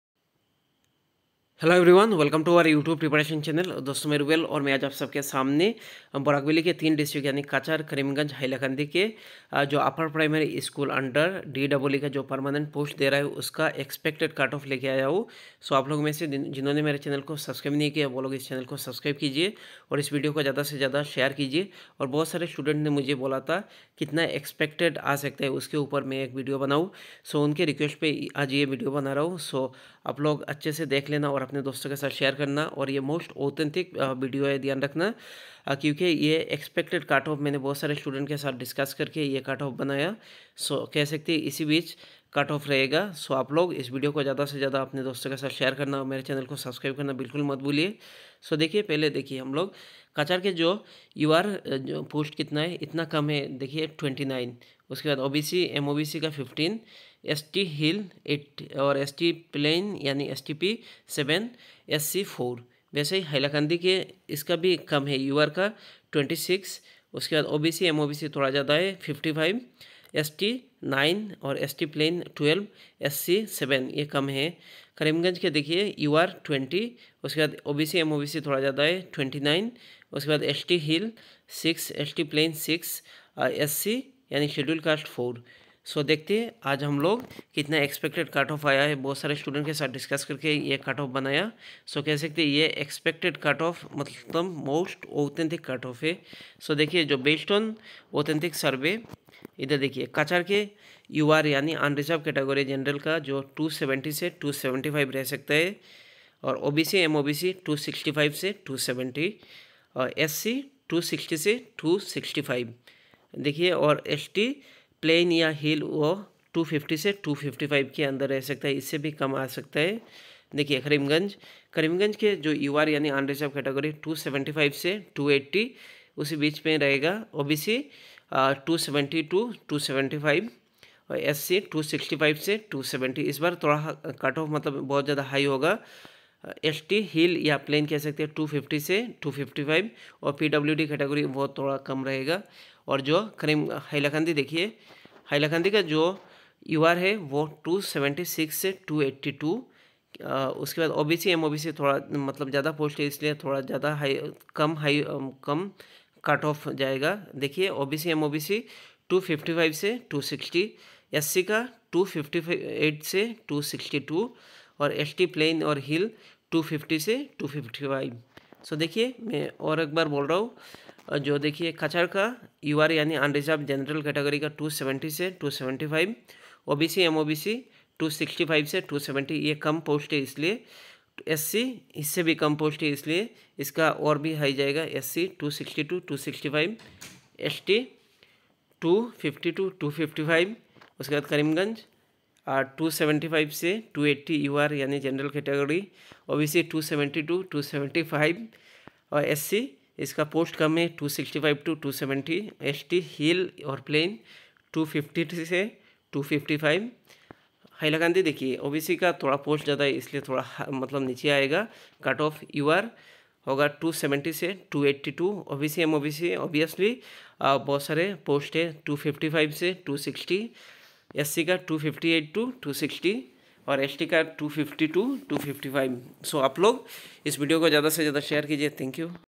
Assam DEE Upper Primary Cut off 2023 for Cachar, Karimganj and Hailkandi cutoffUPS barak valley DEE
cut off for barak valley Dee UP teacher 2023